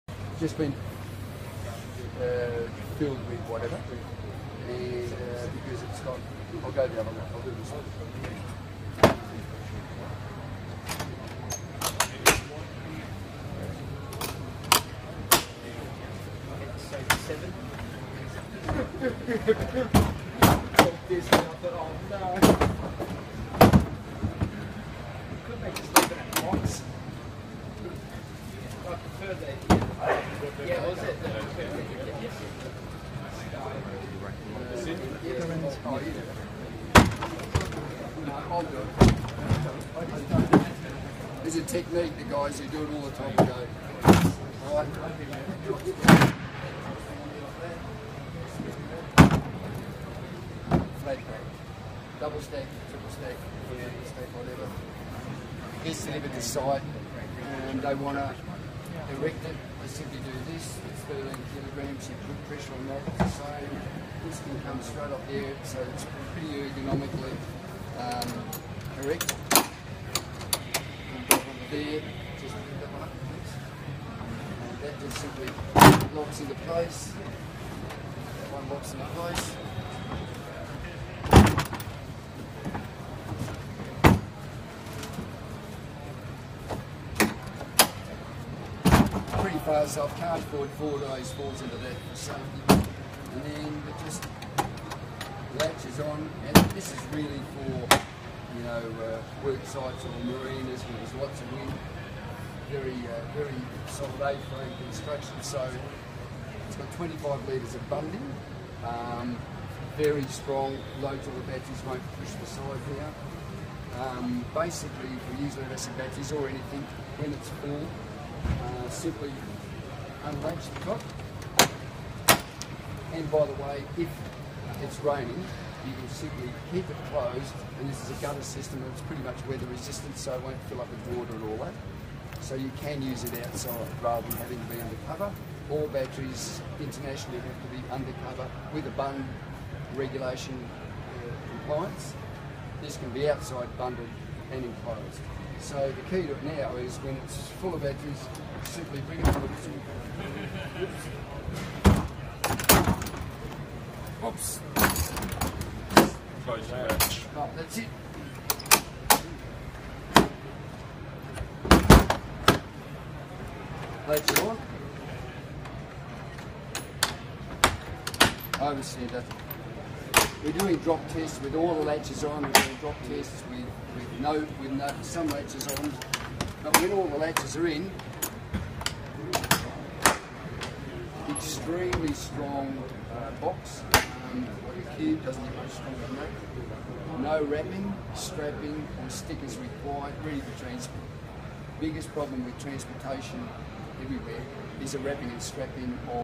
UNISEG Pallet Demonstration By Fenton Goddard
Fenton Goddard, the inventor of the UNISEG Pallet, demonstrating it's feature and versatility at the CeMAT conference in Sydney, May 2015